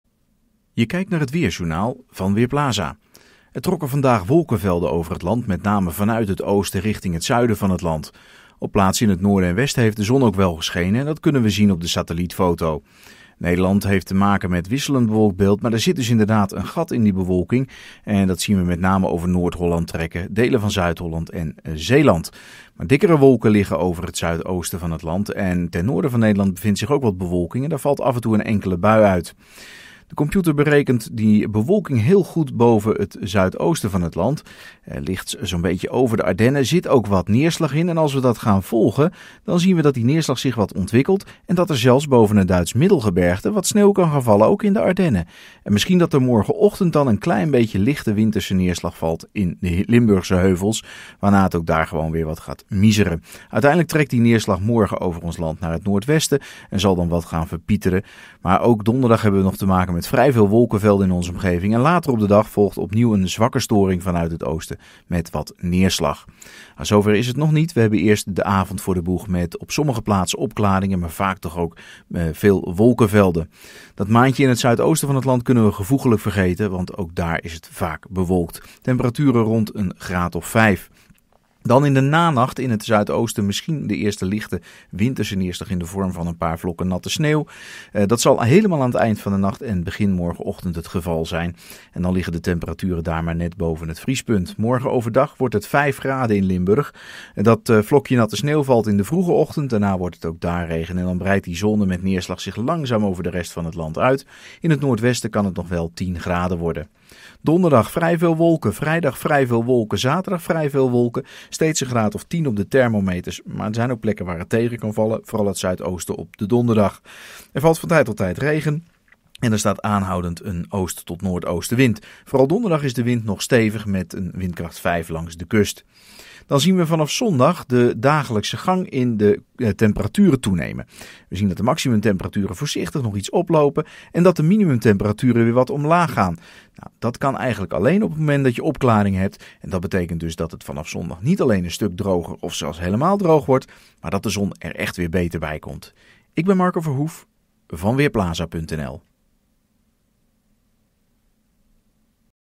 Weerbericht dinsdagmiddag: Kil herfstweer
Vanavond en vannacht valt in het noordwesten wat buiige regen. In het midden van het land zijn er een aantal opklaringen en kan er lokaal wat mist voorkomen. Tegelijkertijd volgt er vanuit het oosten een gebied met neerslag waaruit in Zuid-Limburg morgenvroeg tijdelijk natte sneeuw kan vallen. Later gaat alles over in regen. Morgen overdag zijn er grote verschillen in temperatuur. In het zuidoosten wordt het 5 graden terwijl het kwik op de Wadden stijgt naar 11 graden.